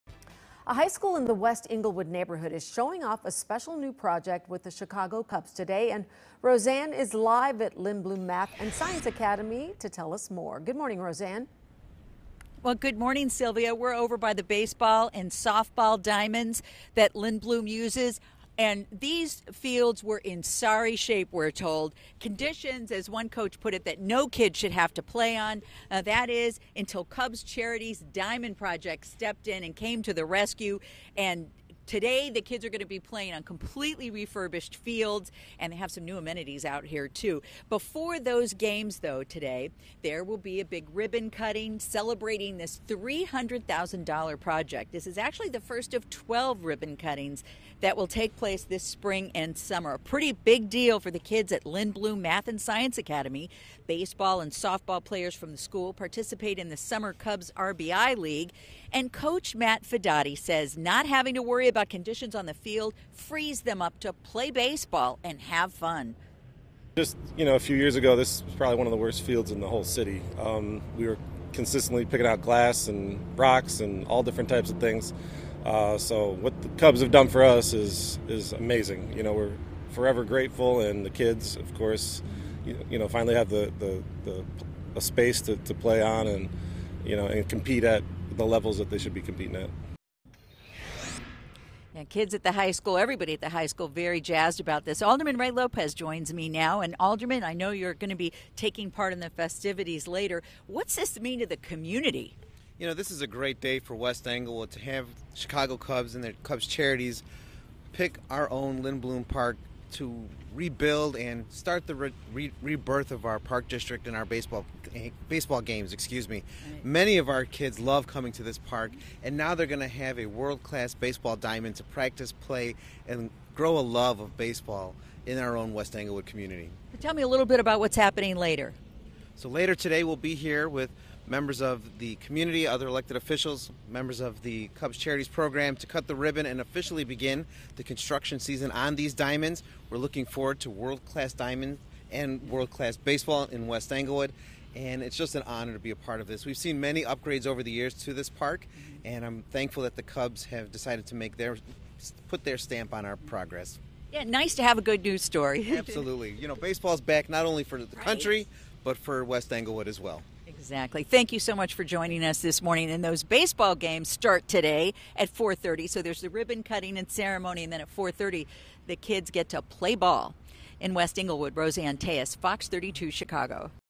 Chicago Cubs help restore dilapidated baseball fields in West Englewood
The Chicago Cubs are hitting the field in spring training in Arizona, but the team is also turning a park district field in Chicago into a new friendly confines for young baseball players